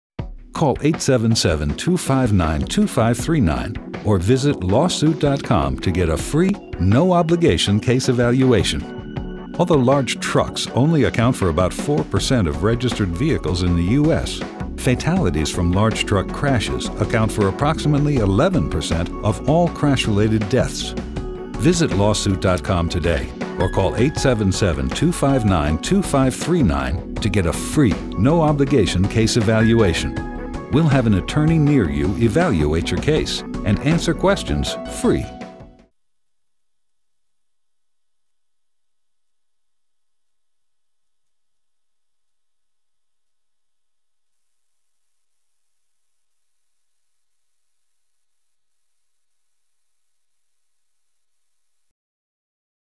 Video - a trucking accident attorney in Los Angeles for you
Did you know that the State of California has had the second highest rate of large truck fatalities in the nation? Have you been the victim of a trucking accident? Attorney ads in Los Angeles seem to be everywhere In a sea of options, you need to make an informed decision when selecting a lawyer.